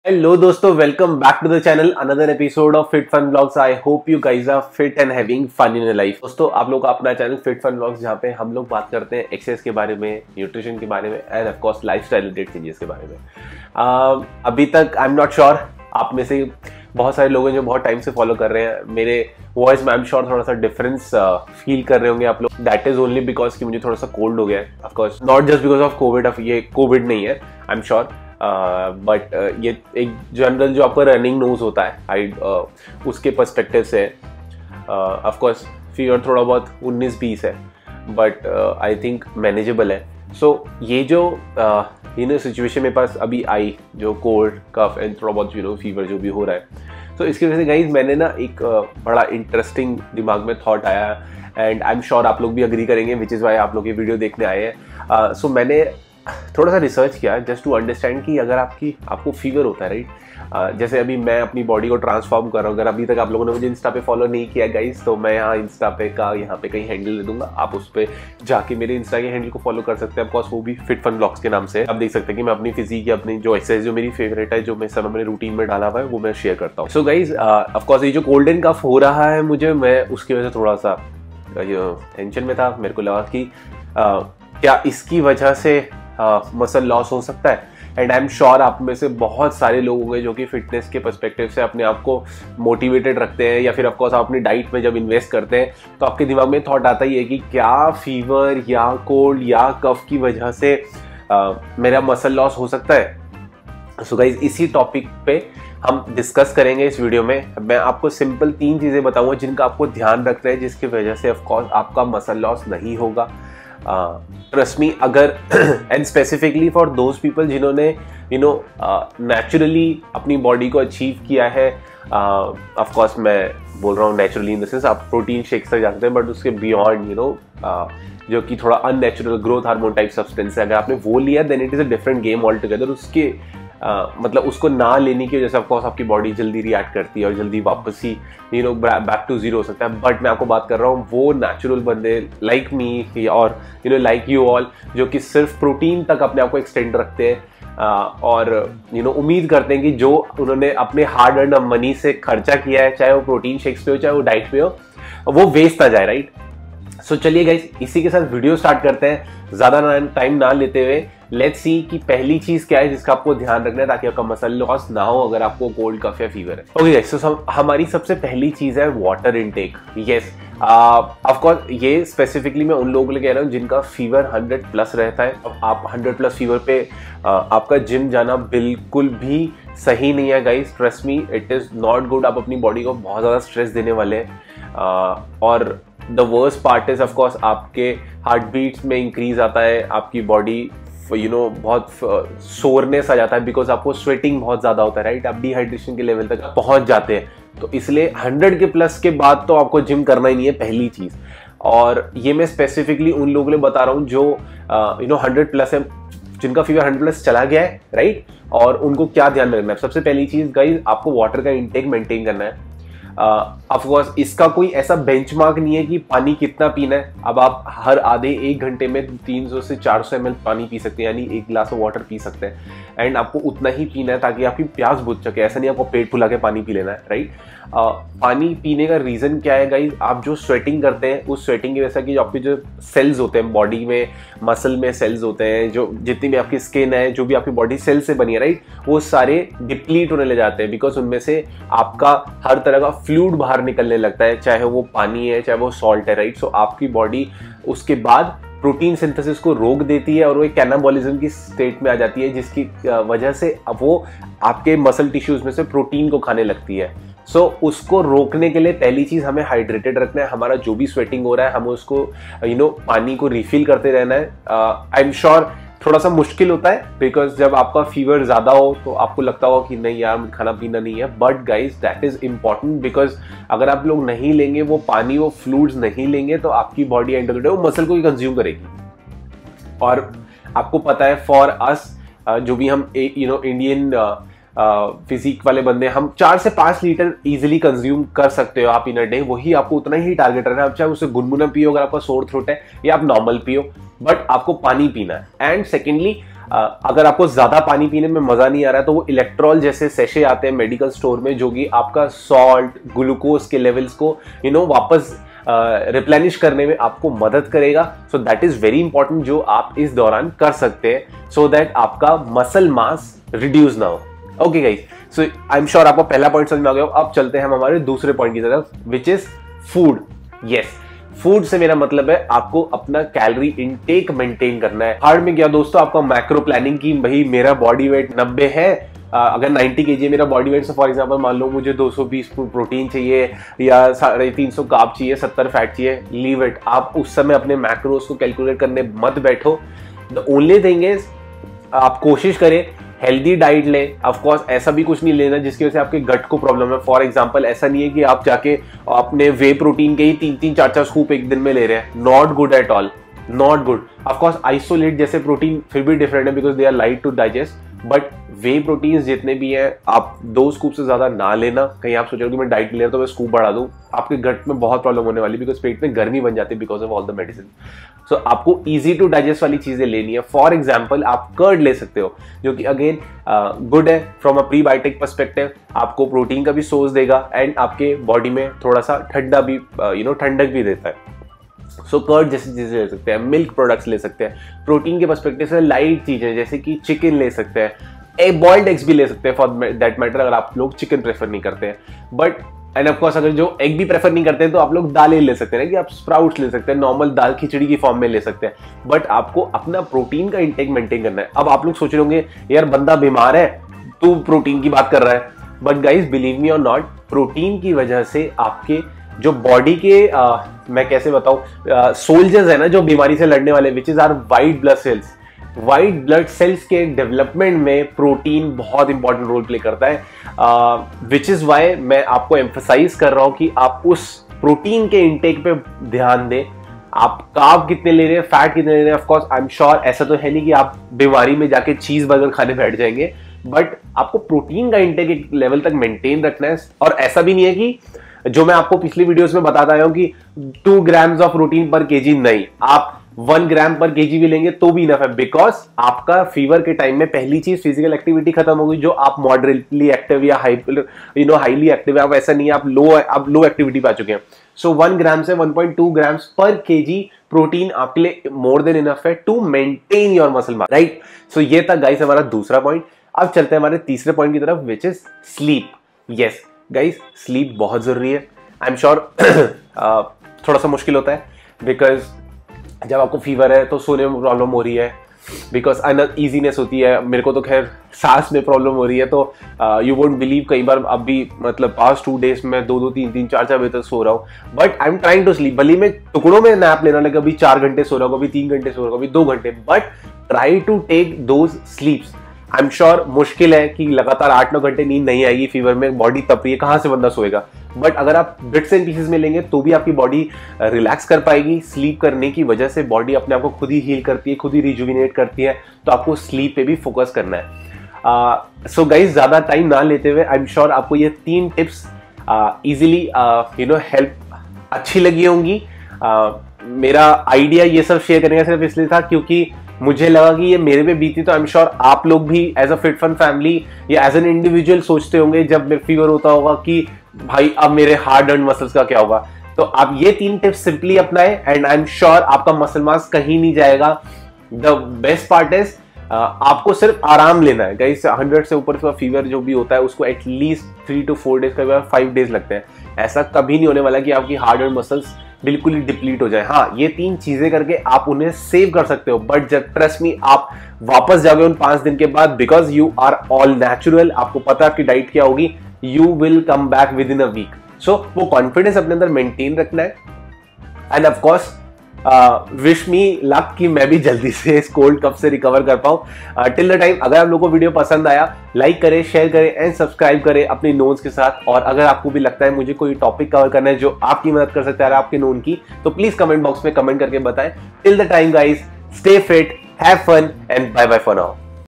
3 Ways to save muscles during fever
3 Ways to save muscles during fever.
Stays fit and have fun in your life!

Gears i used :
Gimble 3
Iphone 11